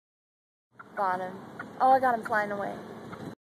Wildlife Wednesday: Episode 2: Wild Turkey
In this video I find some wild turkeys in my friend's driveway. All images and audio are original.